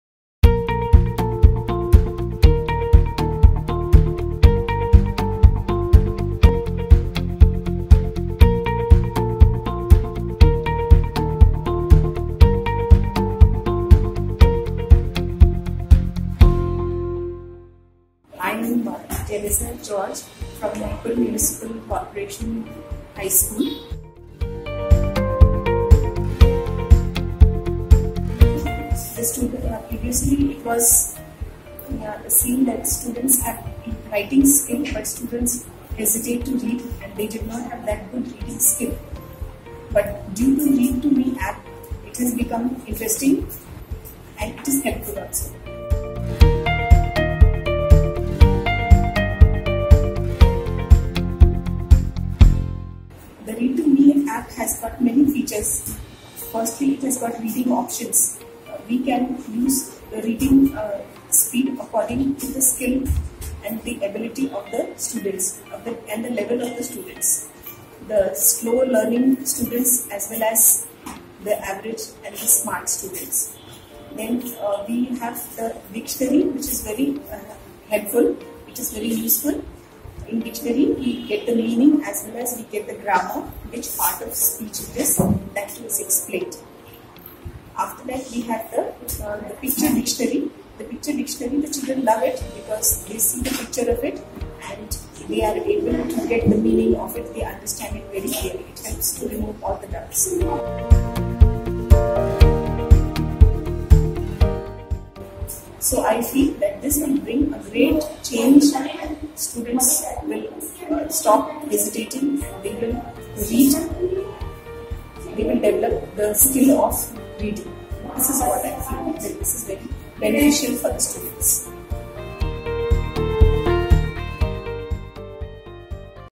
Dr. B. R. Ambedkar Madhyamik School | Nagpur, Maharashtra | ReadToMe® Class | RightToRead
There are three simple steps of Reading with ReadToMe® AI multi-sensory software:

1. Listen: Students listen to the text read by the AI software - ReadToMe®
2. Listen and Repeat: Students then read aloud repeating after the voice
3. After practicing Steps1 and 2, students then read aloud on their own after the teacher mutes the voice from the software

ReadtoMe® enables reading of the prescribed text book.